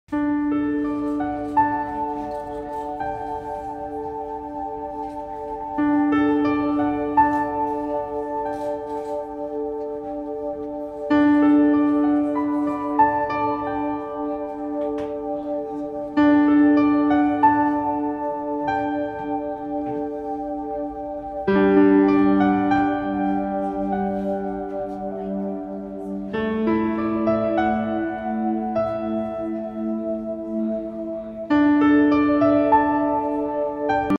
Bedroom Dresser Designs and Ideas | IKEA Style | Bedroom Decor
Back to the Simple one, Style your Bedroom Chest of Drawers This Way.
You can’t go wrong by placing a mirror above a dresser. However, if you already have a mirror somewhere else in the room, you can hang framed art instead.